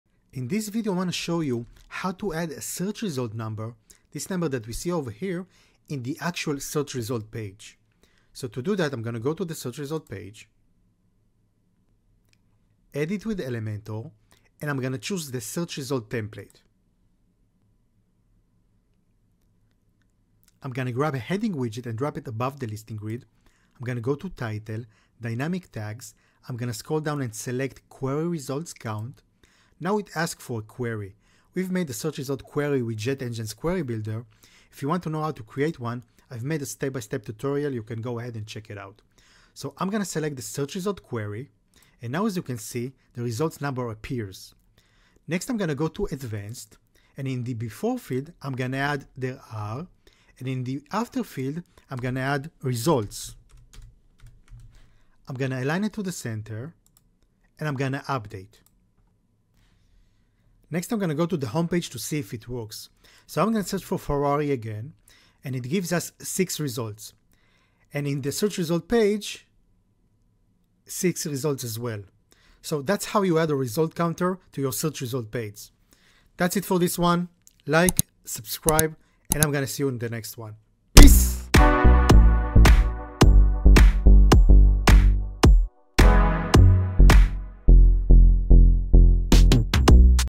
How To Add A Search Result Count To Your Results Page (jet search, jet engine)
in this video im going to show you how to add aresult count to your result page using jet engine and jet search
how to make a search result query: